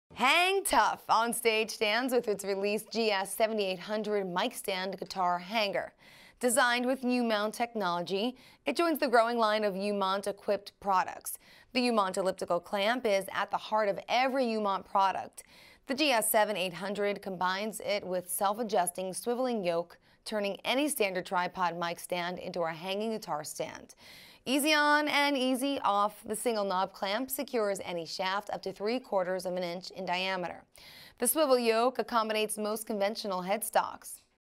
GS7800 u-mount Mic Stand Guitar Hanger
Keep your guitar close! Hang it from your microphone stand. The GS7800 takes advantage of the patent-pending u-mount clamp and lets your existing mic stand pull double duty as a guitar stand. The easy-on, easy-off single knob clamp secures to any shaft up to ¾" in diameter. Your guitar will always be in reach and your piece of the stage will be clutter-free. The swivel yoke fits most conventional headstocks and is lined with inert velveteen foam. An exclusive EVA bumper is included to stabilize and protect your electric or acoustic guitar.